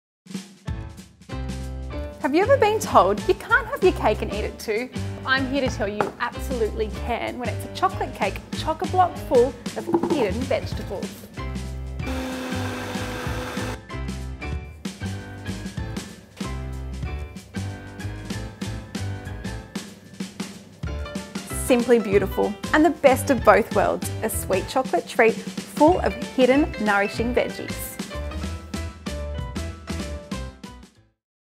HOW TO MAKE AND STYLE FOR PHOTOGRAPHY - ZUCCHINI CHOCOLATE CAKE
Elli Vernon food editor of Better Homes & Gardens magazine will guide you step by step on how to  make this delicious Zucchini Chocolate cake.
and see how quick your food styling takes off to a pro level.